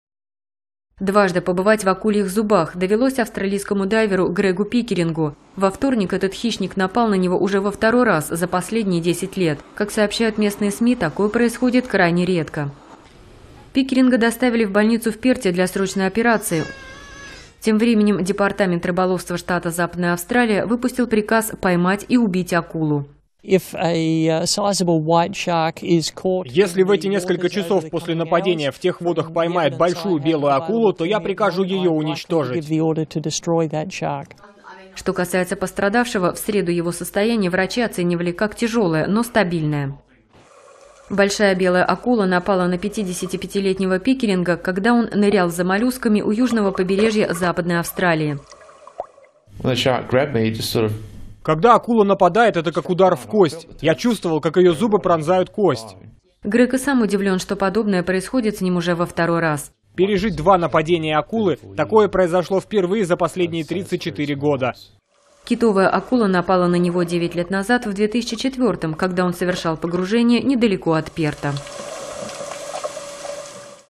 Дайвер пережил нападение акулы уже во второй раз (новости)
Дайвер пережил нападение акулы уже во второй раз. Дважды побывать в акульих зубах довелось австралийскому дайверу Грегу Пикерингу. Во вторник этот хищник напал на него уже во второй раз за последние 10 лет. Как сообщают местные СМИ, такое происходит крайне редко.

Пикеринга доставили в больницу в Перте для срочной операции.

Тем временем Департамент рыболовства штата Западная Австралия выпустил приказ поймать и убить акулу.

[Стюарт Смит, глава Департамента рыболовства штата]:
«Если в эти несколько часов после нападения в тех водах поймают большую белую акулу, то я прикажу ее уничтожить».

Что касается пострадавшего, в среду его состояние врачи оценивали как тяжелое, но стабильное.

Большая белая акула напала на 55-летнего Пикеринга, когда он нырял за моллюсками у южного побережья Западной Австралии.

[Грег Пикеринг, пострадавший дайвер]:
«Когда акула нападает -- это как удар в кость. Я чувствовал, как ее зубы пронзают кость».

Грег и сам удивлен, что подобное происходит с ним уже во второй раз.

«Пережить два нападения акулы -- такое произошло впервые за последние 34 года».

Китовая акула напала на него 9 лет назад, в 2004-м, когда он совершал погружение недалеко от Перта.